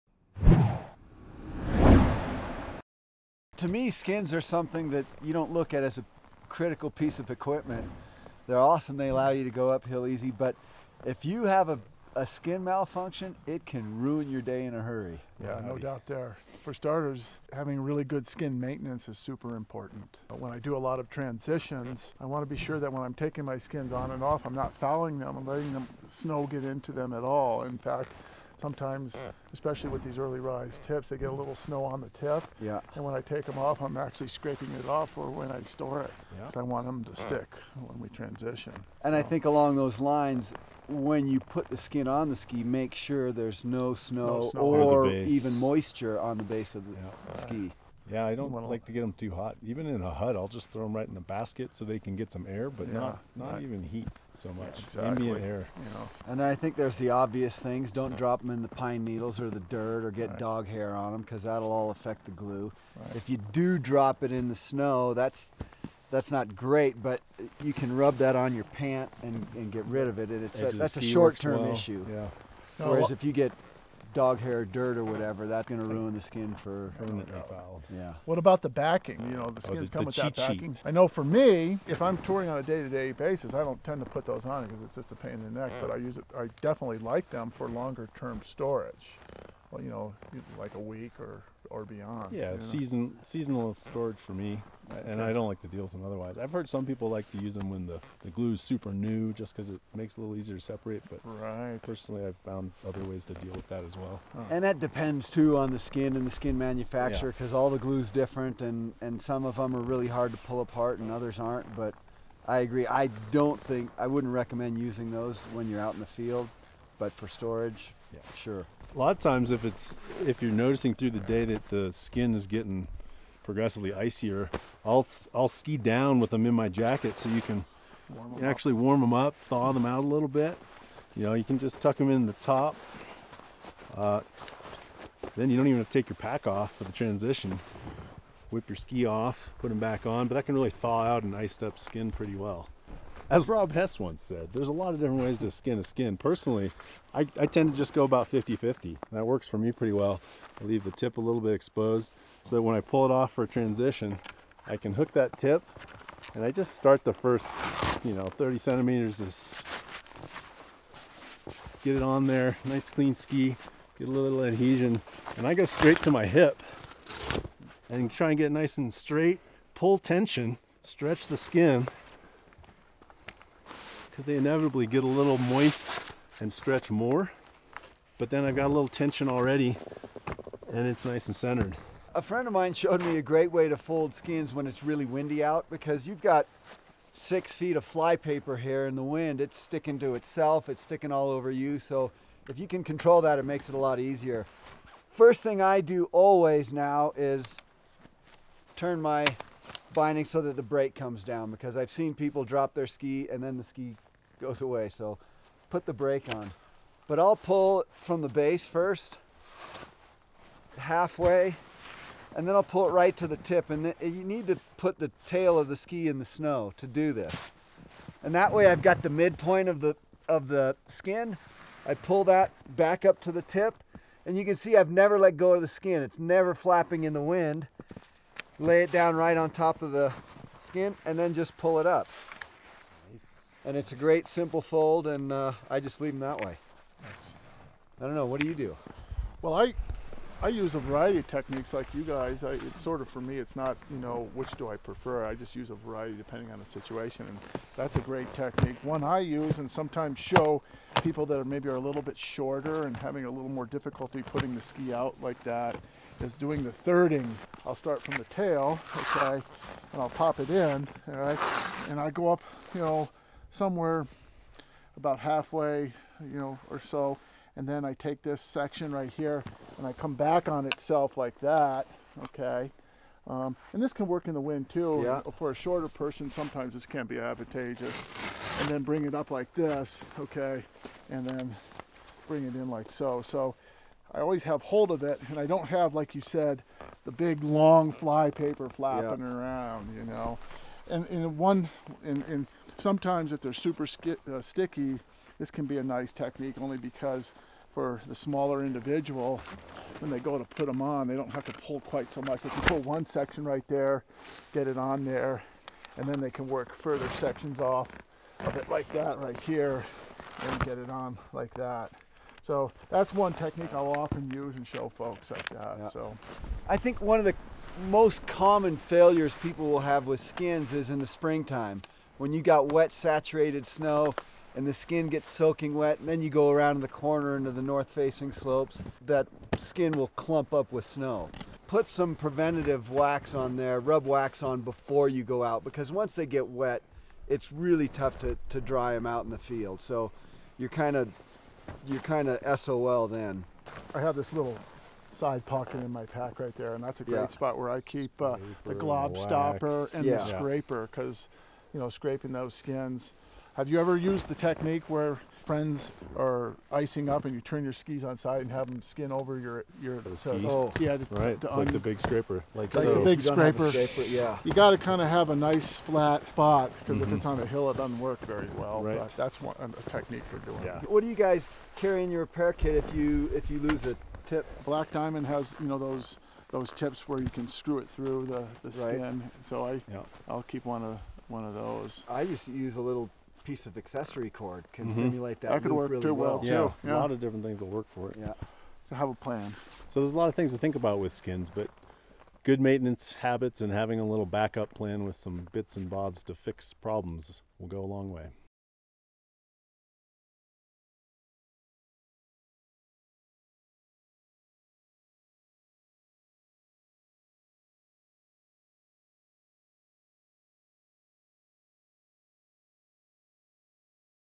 How to Care for Your Climbing Skins While Backcountry Skiing
Learn how to care for your climbing skins with instruction from the American Mountain Guides Association.